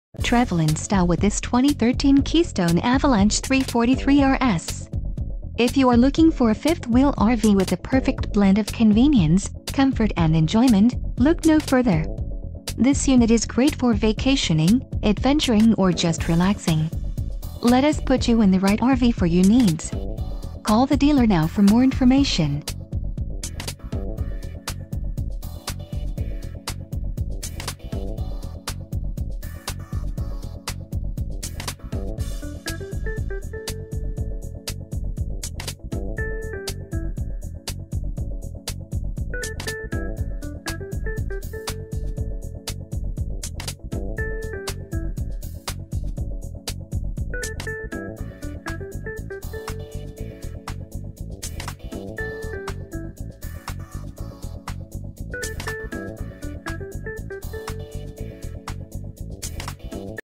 2013 Keystone Avalanche 343RS - Chilhowee RV Center - Lou...
This 2013 Avalanche fifth-wheel trailer by Keystone is the model 343RS.